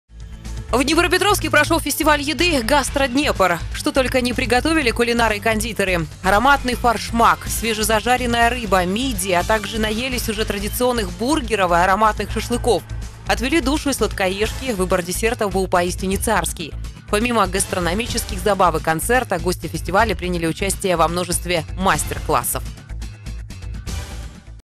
Сладкоежки отвели душу (15.10.2015)
Как прошел фестиваль еды
В Днепропетровске прошел фестиваль еды Gastro Dnepr. Чего только не приготовили для гостей кулинары и кондитеры!